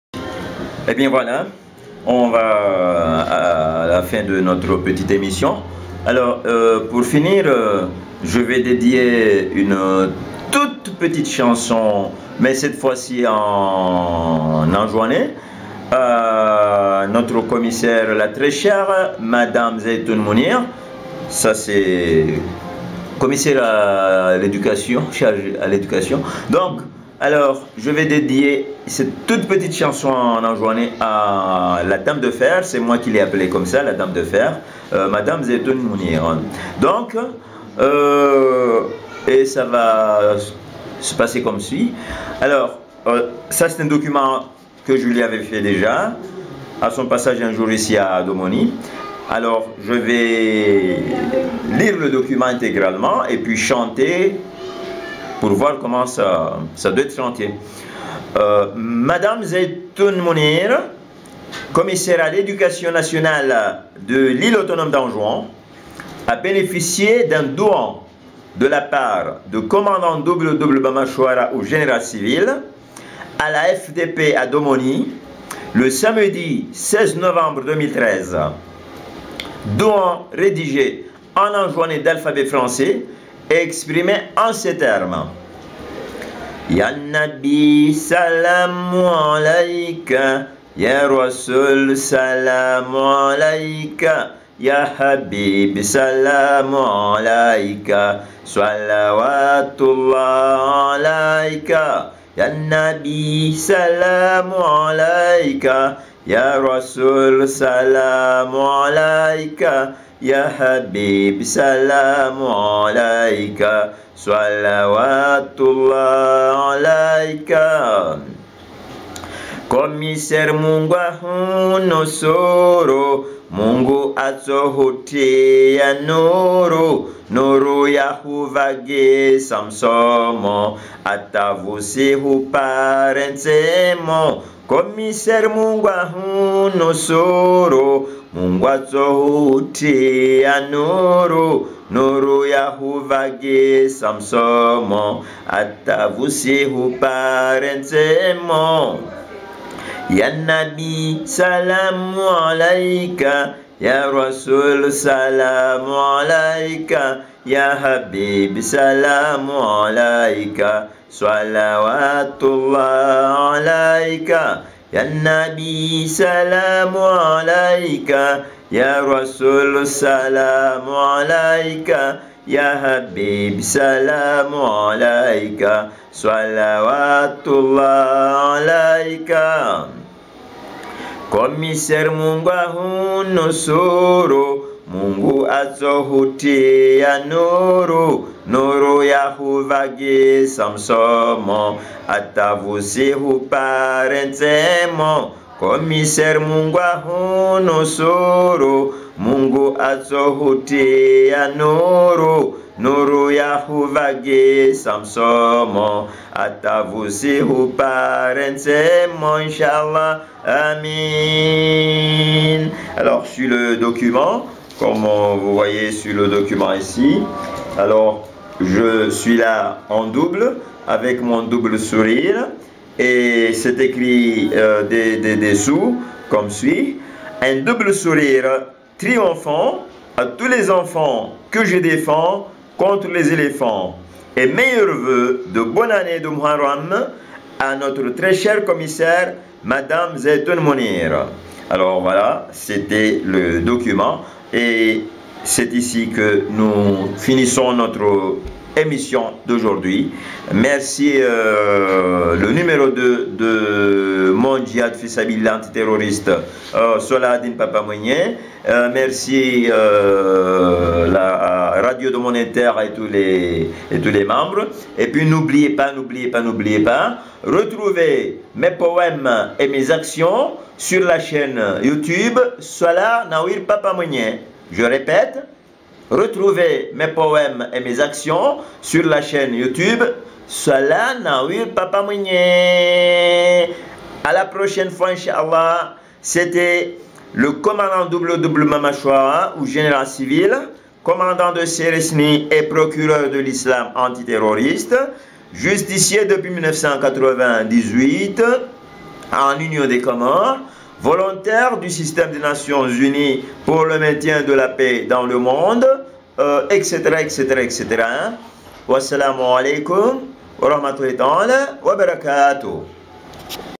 Bamachouwara pour commissaire Zaitoune Mounir
Le poète acharné, Général civil nous interprète un petit poème pour le dédier à Madame la commissaire en charge de 'éducation à Anjouan, Zaitoune Mounir. Très bonne écoute les Fans..